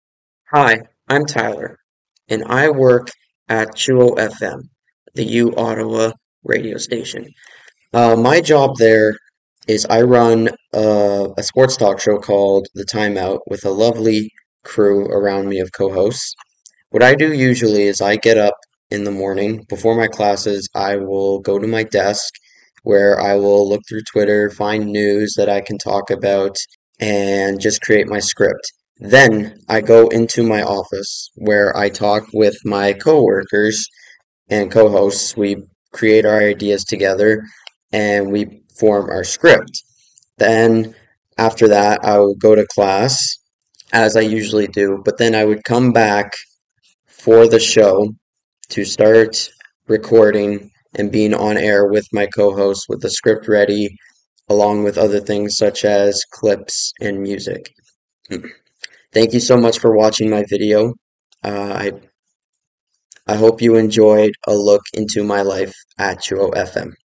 My life at CHUO
In this video, I will take you through my average day at CHUO FM as a Sports talk show host